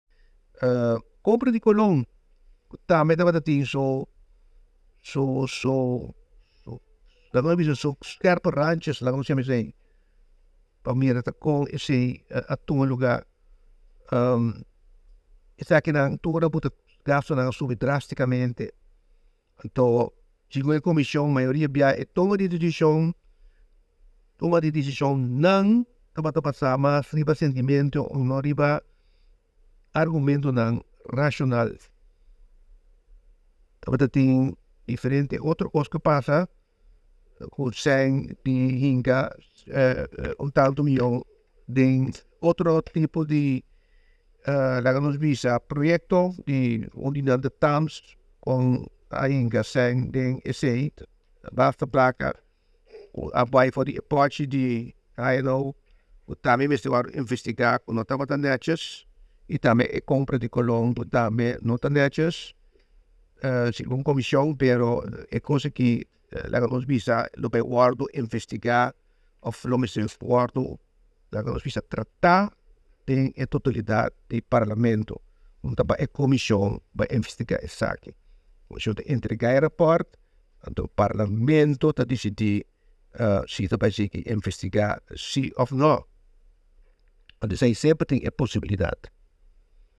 Toma di desishonan tabata basa mas riba sintimentu
Sigun e komishon di enkuesta HNO mayoria biaha e toma di desishonan tabata basa mas riba sintimentu i no riba argumentunan rashonal.